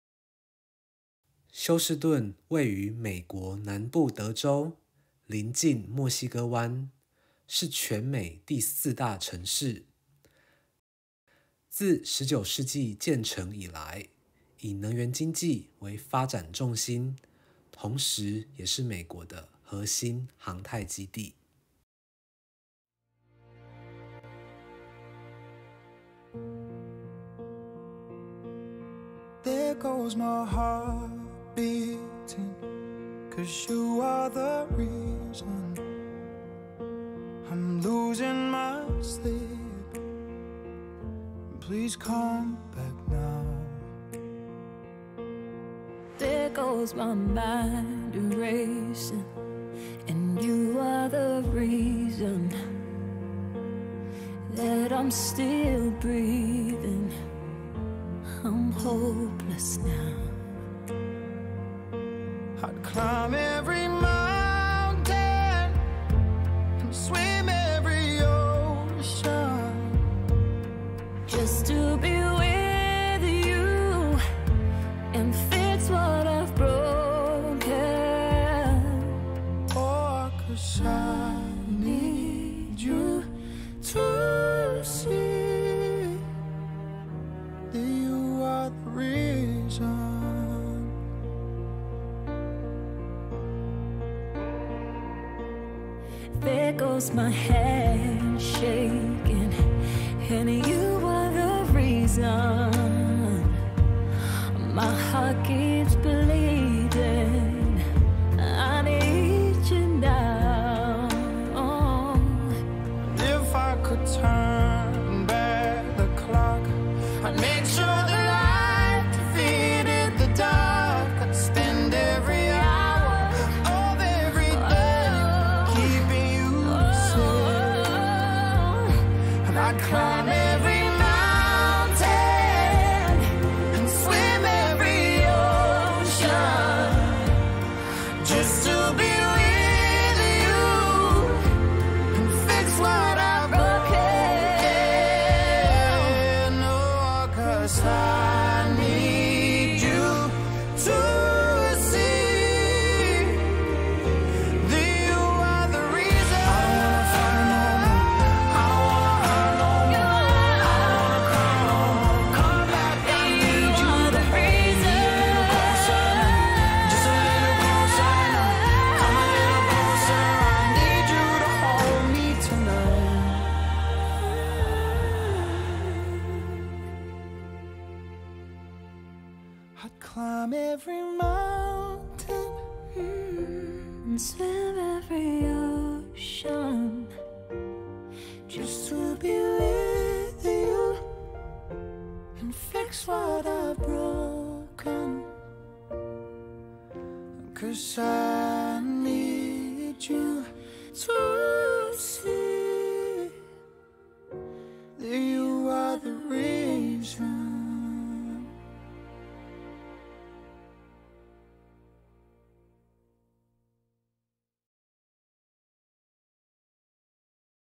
Travel | 德州休士頓 Houston Texas
影片場景
Houston Graffiti Building
Sam Houston Statue at Hermann Park
Pioneer Memorial Obelisk
Mary Gibbs and Jesse H. Jones Reflection Pool
Hermann Park Lake Plaza
Rice University
Museum District
The Menil Collection
Rothko Chapel
Cullen Sculpture Garden
Minute Maid Park
Toyota Center
Buffalo Bayou
Gerald D. Hines Waterwall Park